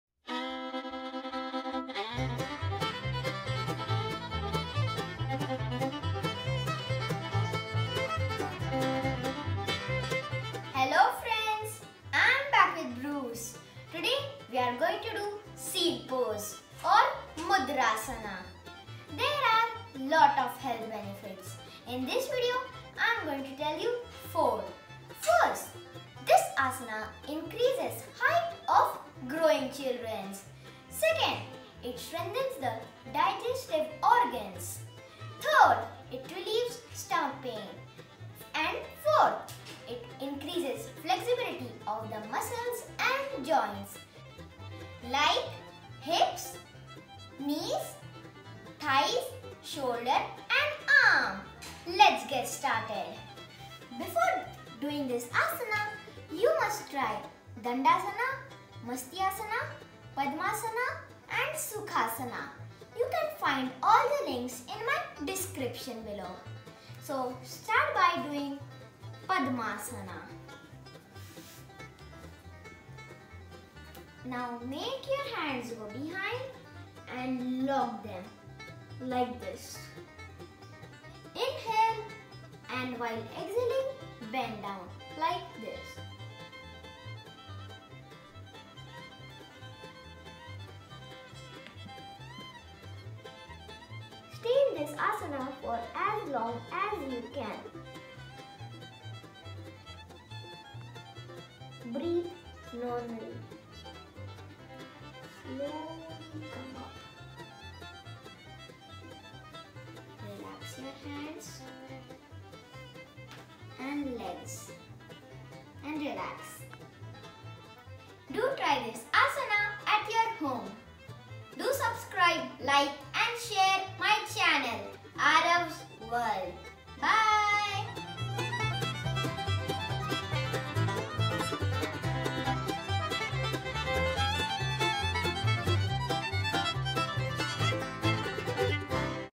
Mudrasana || Yoga Seal pose
Benefits:
- Reduces Stomach Pain
- Helps Strengthens the Digestive Organs
- It can also Grow the Height of Growing kids if one practice it Daily
- Stretches the Muscles & Joints around Hips, Thighs, Knees, Shoulders, Arms
- Gives More Flexibility to Muscles & Joints Like Knees, Shoulders, Arms, Thighs and hips